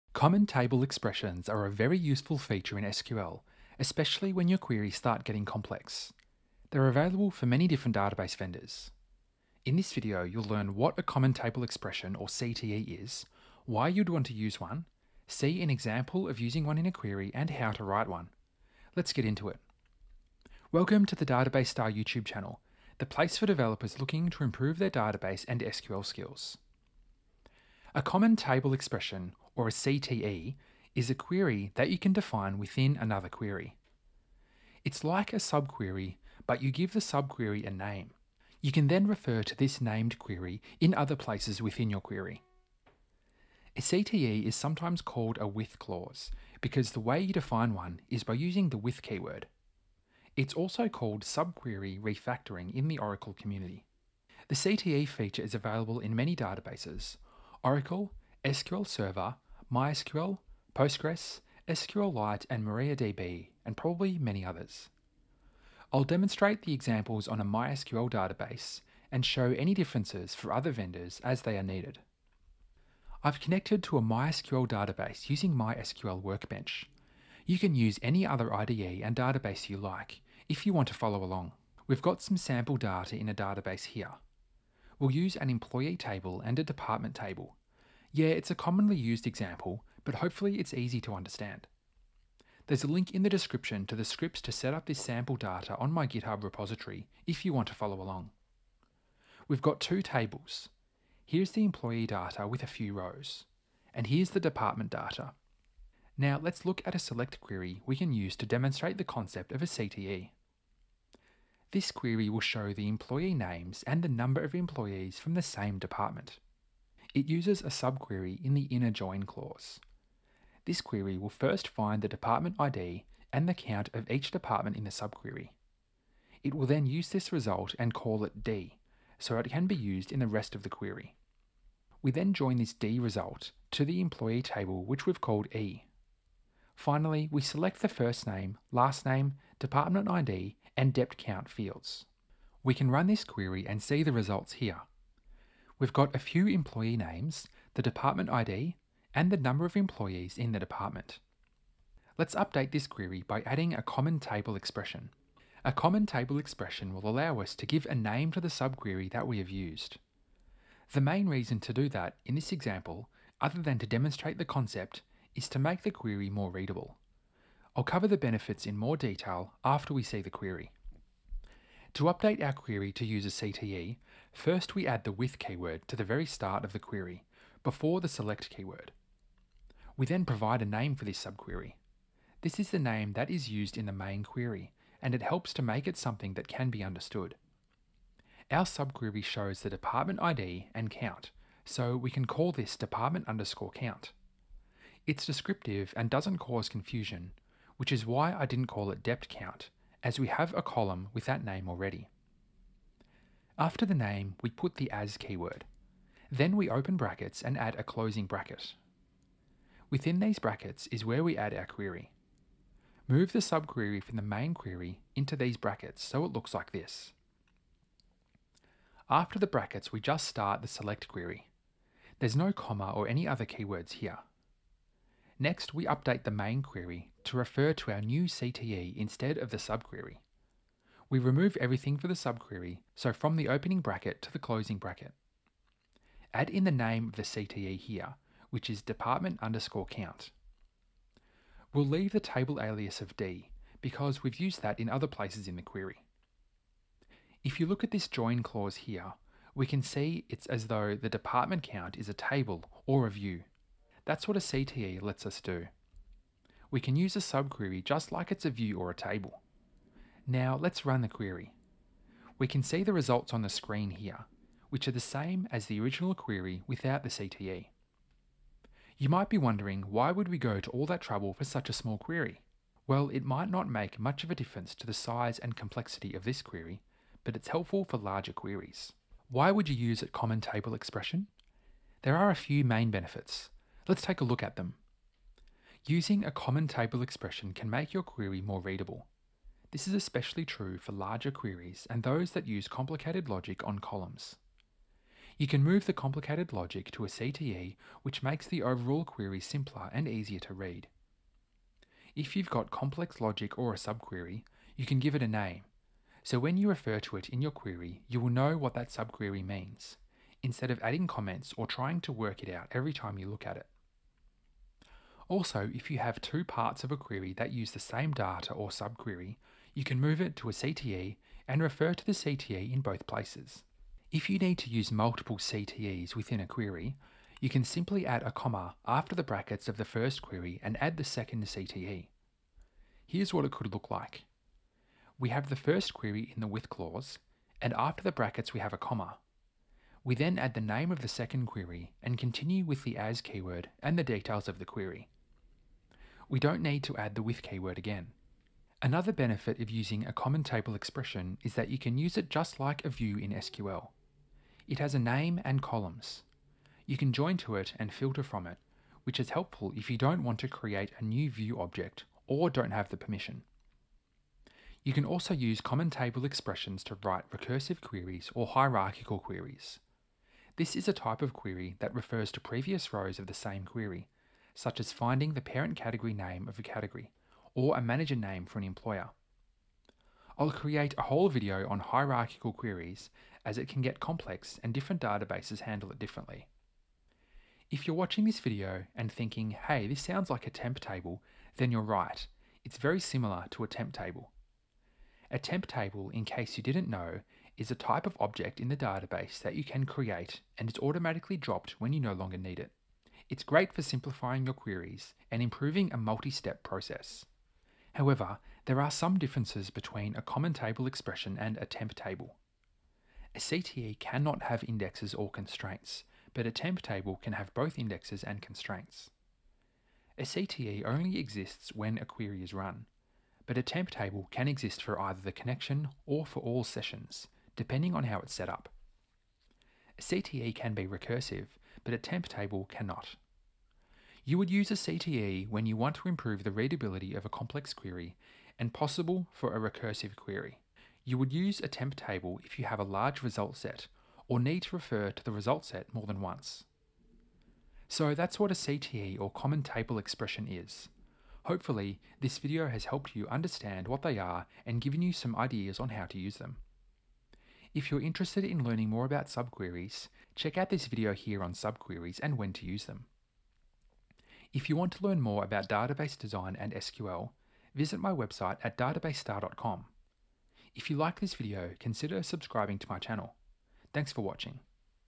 SQL CTEs (Common Table Expressions) - Why and How to Use Them
SQL CTEs, or Common Table Expressions, are a helpful feature in SQL. They can help you clean up a query and make it easier to read, and are available in many vendors including Oracle, SQL Server, MySQL, and Postgres.

In this video, you’ll learn what a CTE is, why you would want to use one, see an example of transforming a query to use a CTE, and see the benefits of a CTE.

Timestamps:
00:00 What is a CTE?
01:13 Our sample data
01:51 Create a CTE
04:38 Benefits of using CTEs
06:27 CTE vs Temp Table